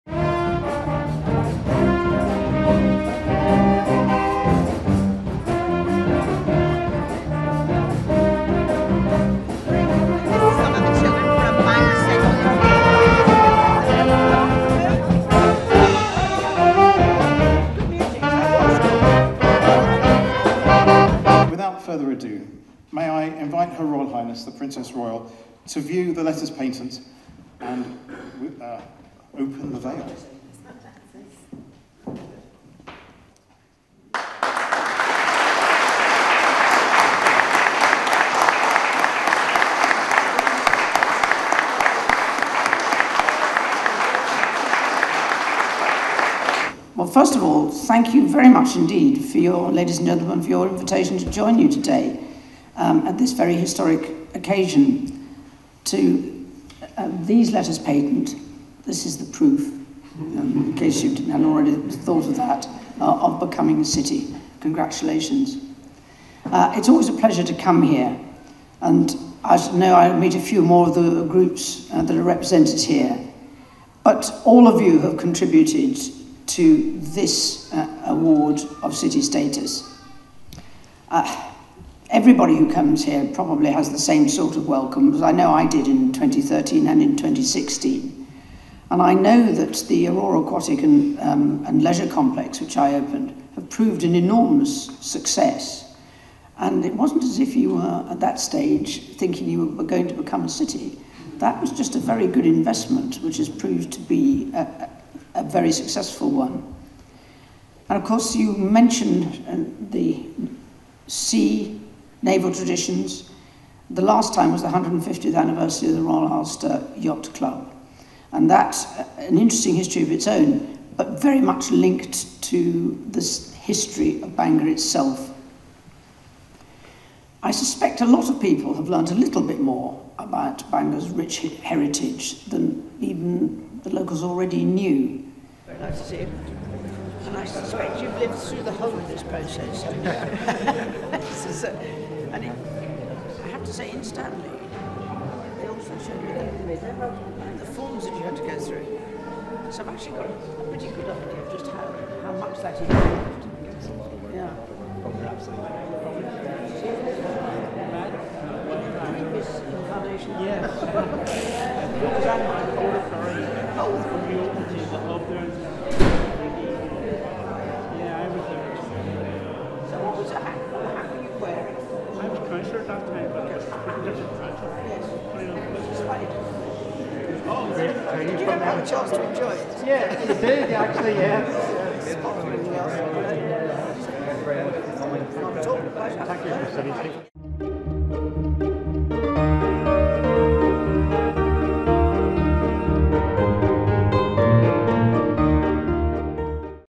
Princess Anne Visits NI as Bangor Receives City Status
Princess Anne attends an official ceremony which sees Bangor in County Down formally receive city status.

During the event, the Princess Royal unveils and views the letters patent and delivers a speech whereby she congratulates the city.

Bangor was one of eight towns that had been awarded city status in May after a competition to mark the platinum jubilee of Queen Elizabeth II.

Report by Roshni Patel.